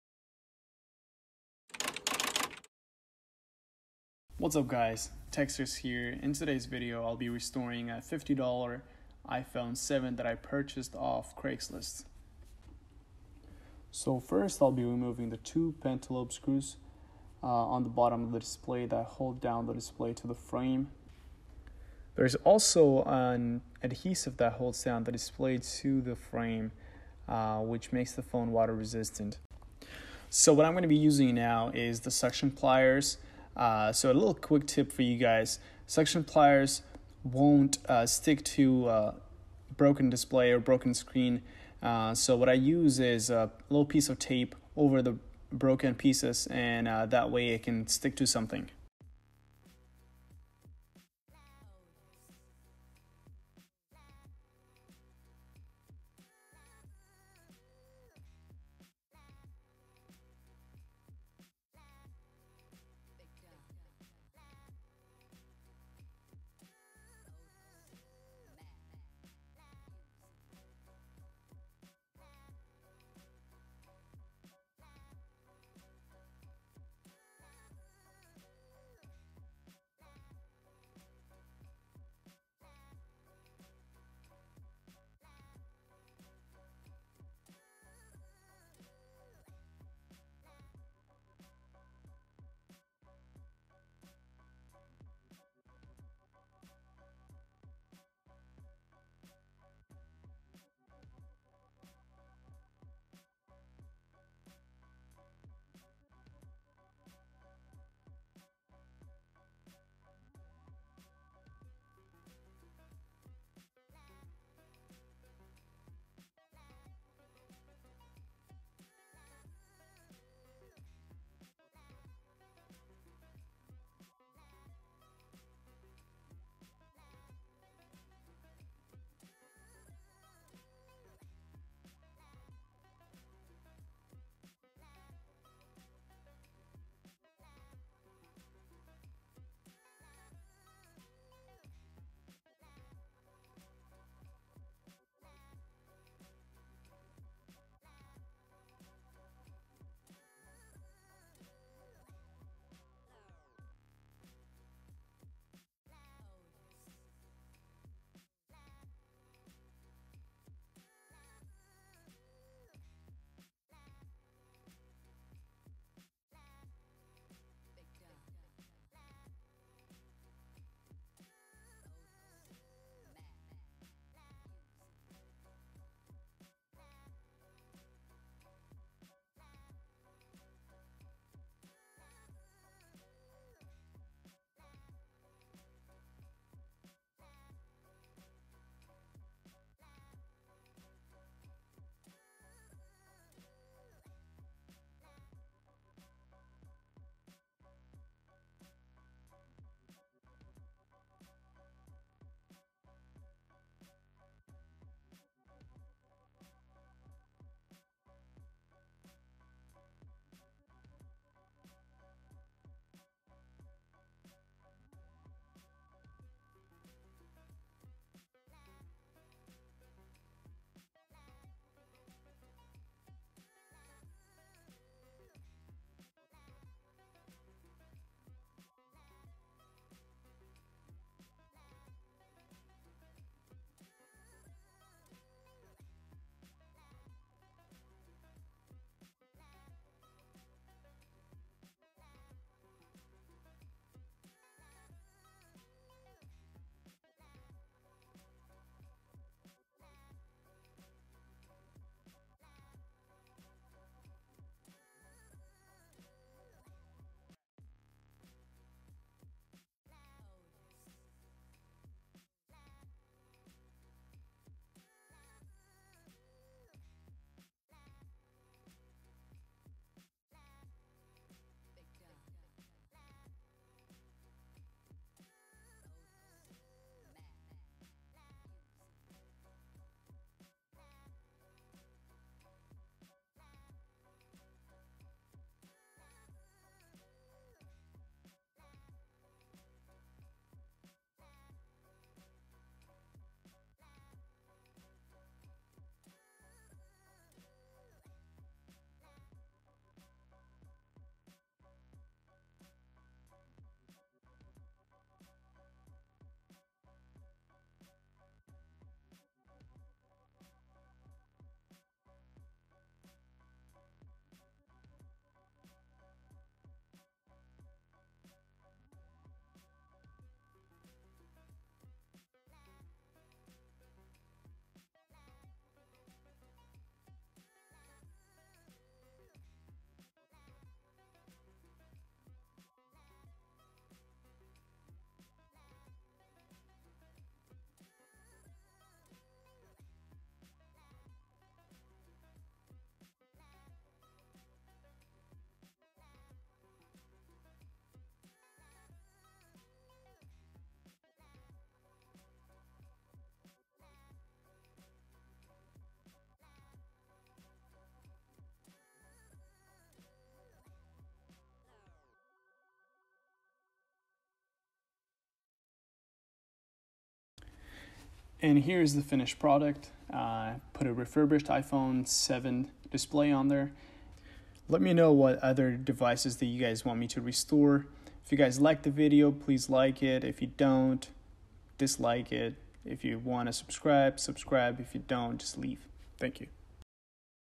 $50 iPhone 7 Restoration
What's up guys I'm Techster, I purchased this iPhone 7 for 30$ and I purchased a refurbished Apple display for 20$ this phone cost me 50$ total. later on I found out that the phone was unfortunately iCloud locked bummer, but it's okay since I have a lot of spare parts from this cheap iPhone 7.